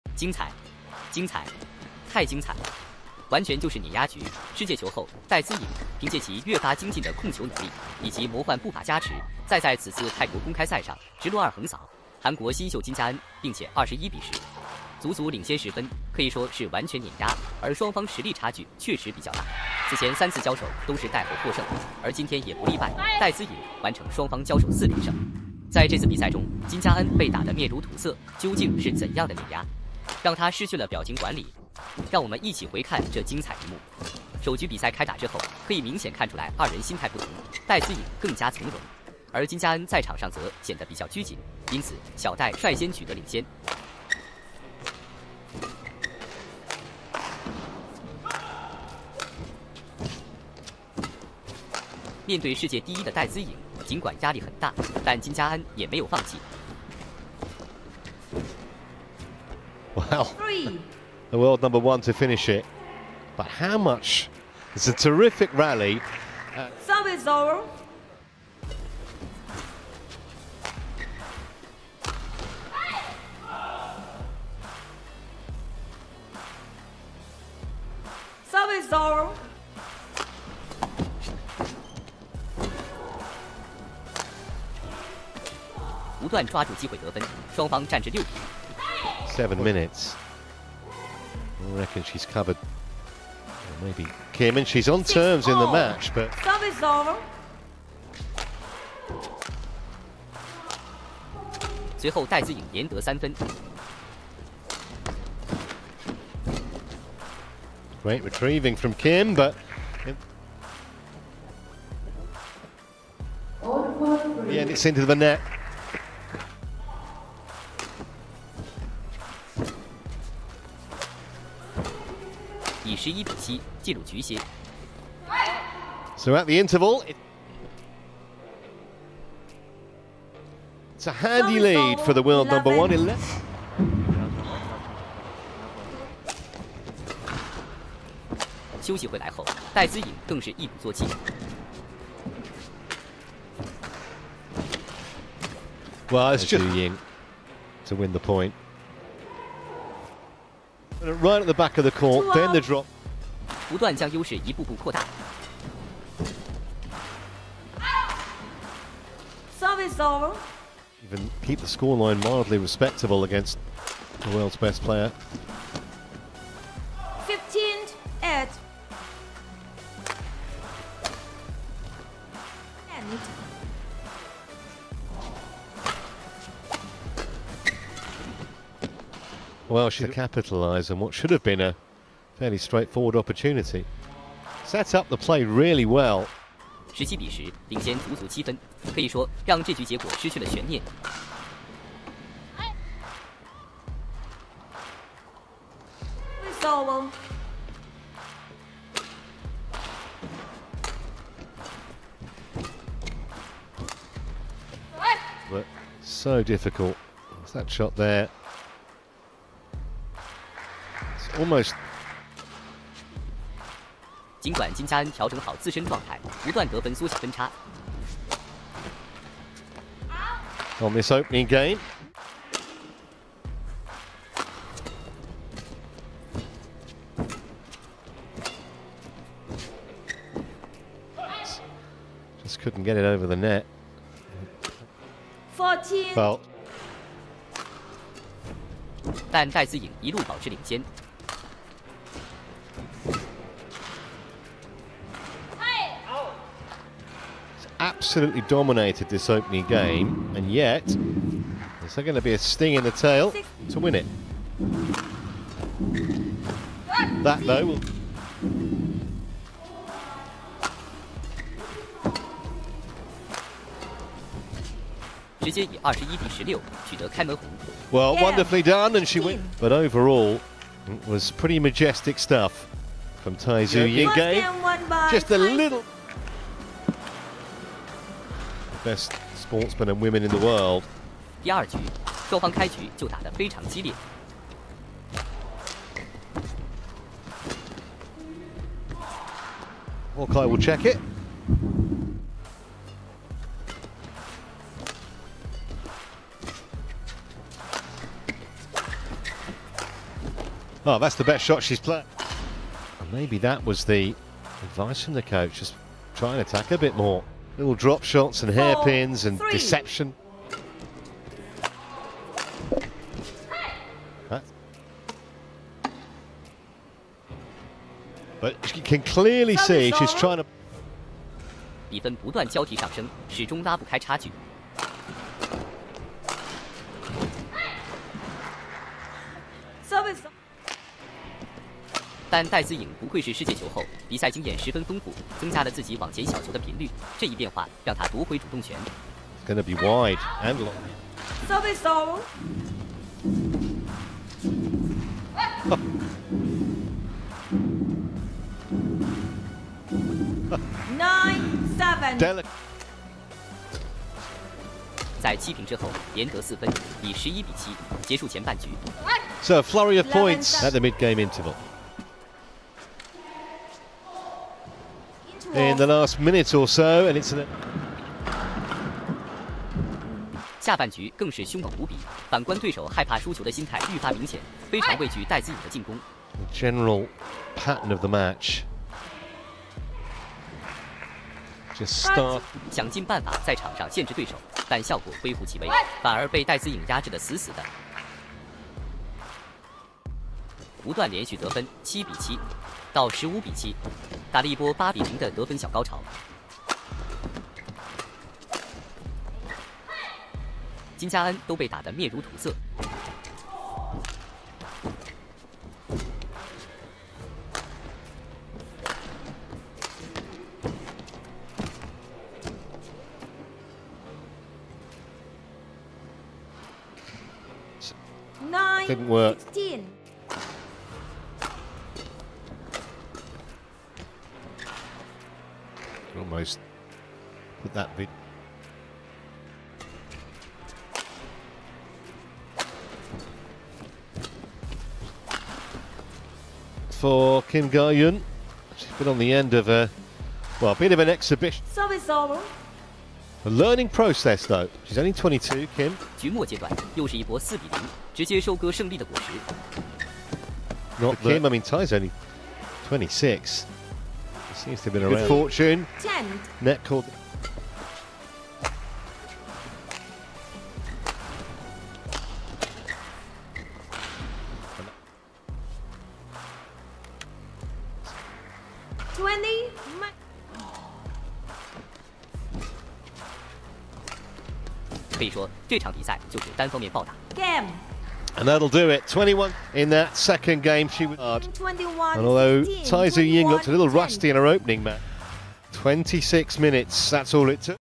【泰國賽 名場面】戴資穎：暴打韓國奪冠功勛！世界冠軍 金佳恩：從比賽一開始，就被她拿捏死死，冷汗直冒，全程一臉痛不慾生的表情... ...
戴資穎 vs 金佳恩||韓國 2017 蘇迪曼杯、2022尤伯杯兩度奪得團體賽，登頂世界冠軍的絕對團隊功勛，卻從職業一開始就淪為戴資穎單打項目的絕對背景帝... ...
從這泰國賽的這次交手，從她一臉痛苦的表情，或許你會讀懂，她被克制的最大原因！